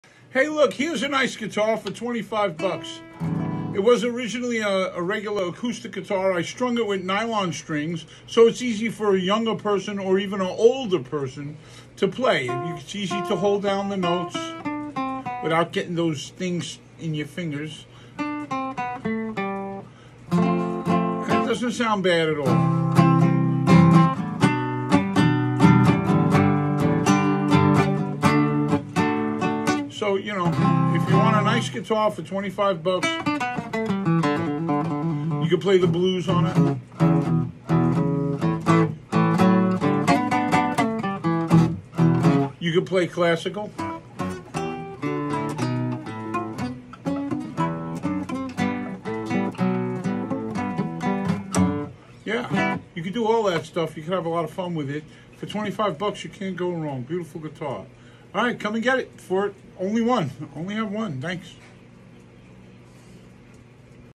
Nice guitar
This is a video of a guitar I’m selling for $25 it’s strong with nylon string so it’s easier to play